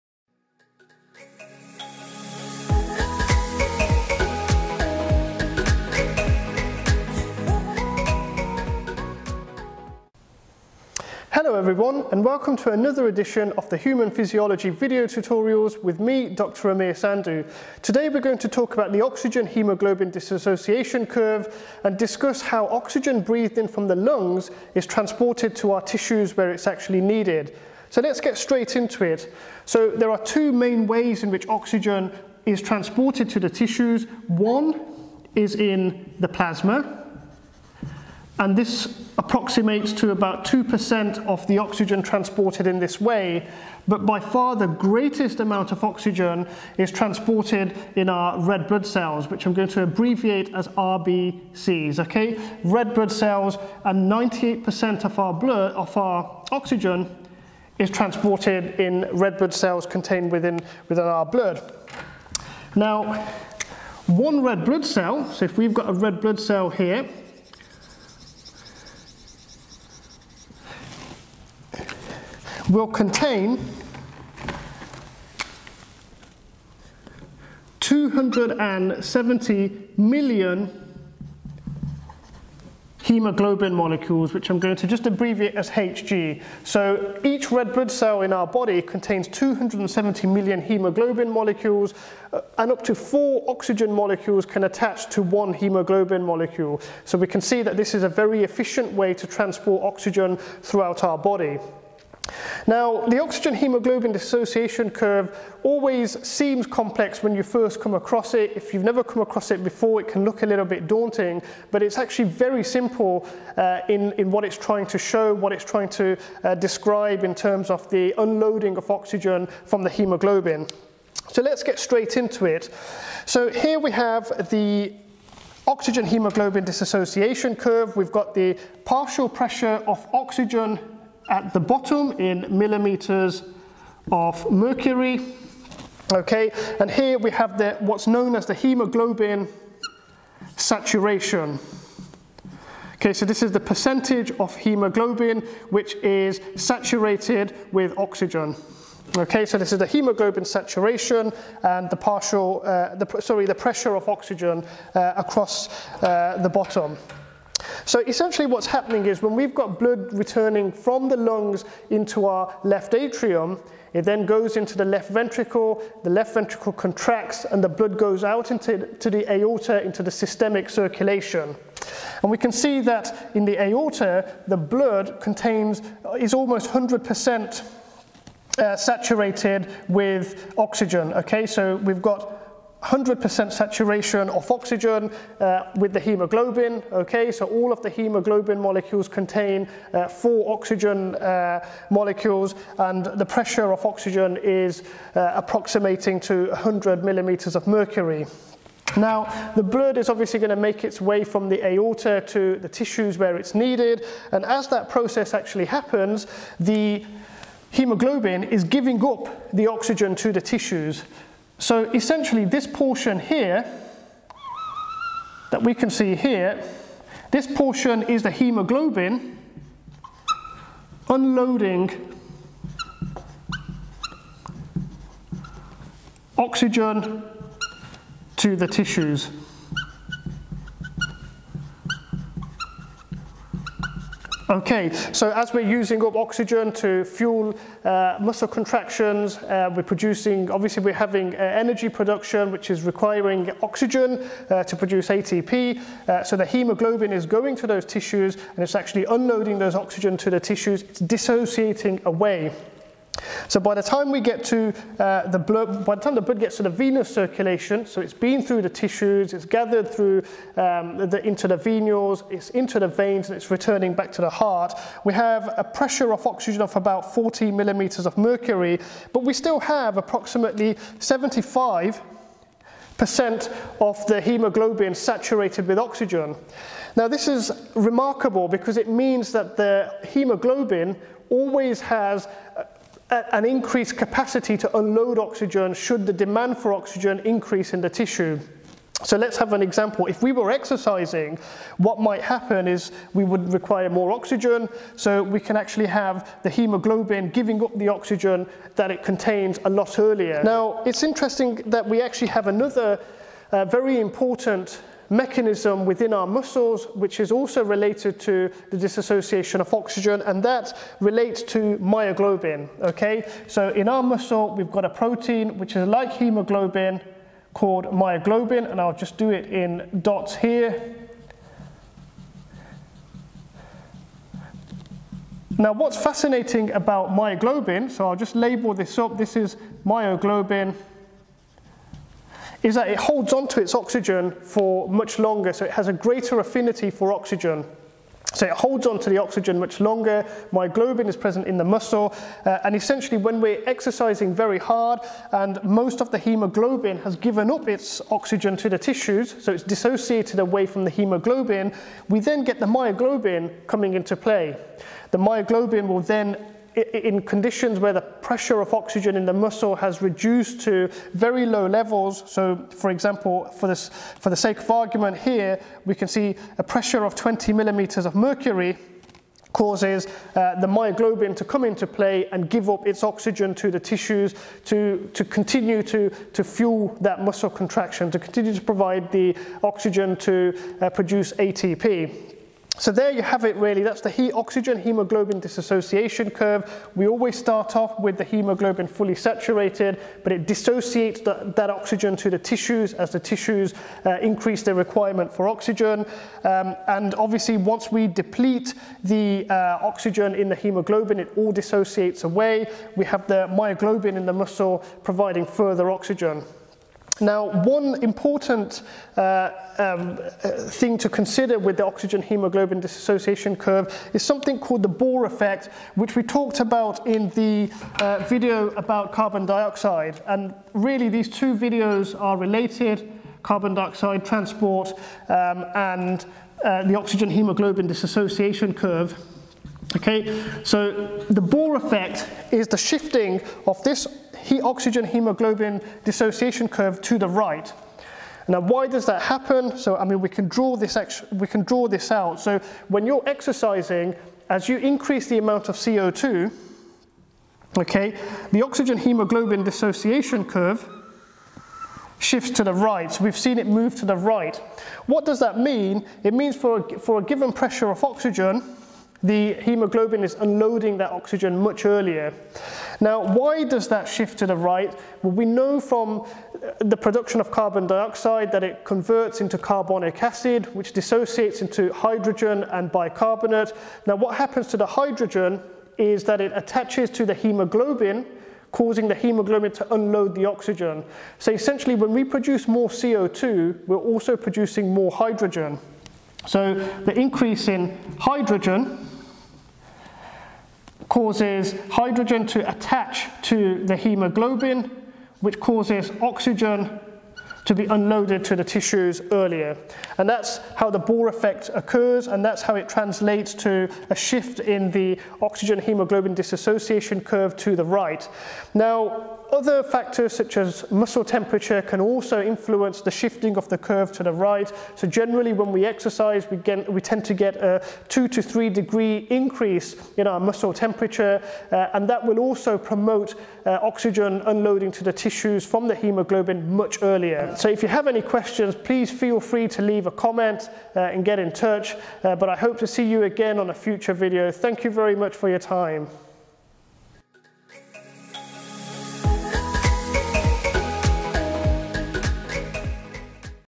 The Oxygen Haemaglobin Dissociation Curve
In this video tutorial, I discuss how oxygen dissociates from haemaglobin according to the oxygen demands of the tissue.  I also describe the Bohr Effect (the shift in the curve to the right when carbon dioxide and muscle temperature is increased).

The video tutorial on carbon dioxide transport may also be helpful in building your knowledge.